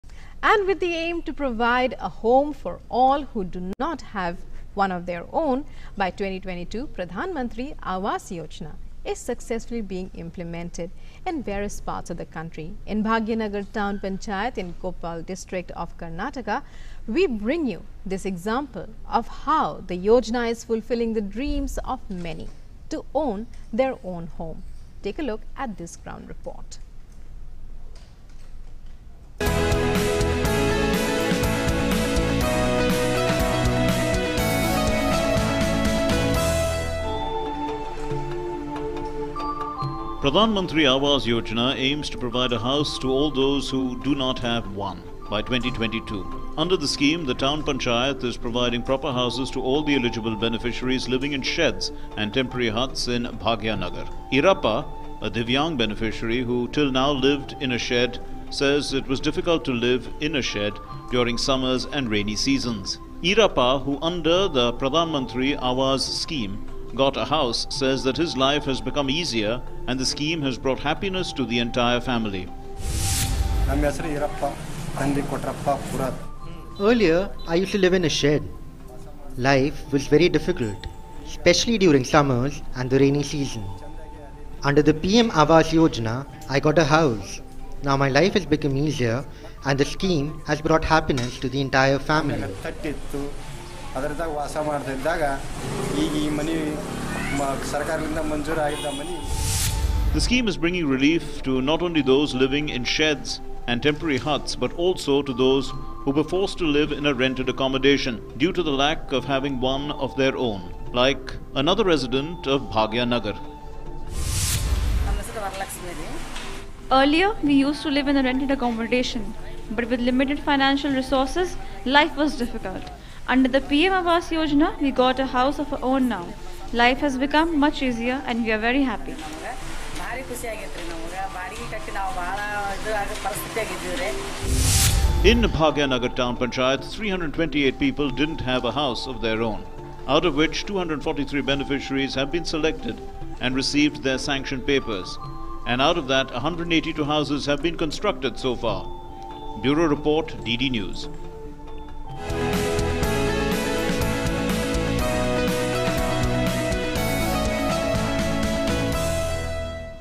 Ground Report | Koppal, Karnataka | PMAY fulfilling dreams of many
With an aim to provide a house to all those who don't have one, by 2022, Pradhan Mantri Awas Yojana is successfully being implemented in Bhagyanagar Town Panchayat in Koppal district of Karnataka. Pradhan Mantri Awas Yojana is fulfilling the dreams of many in Bhagyanagar Town Panchayat. Let's have a look at the ground report from there.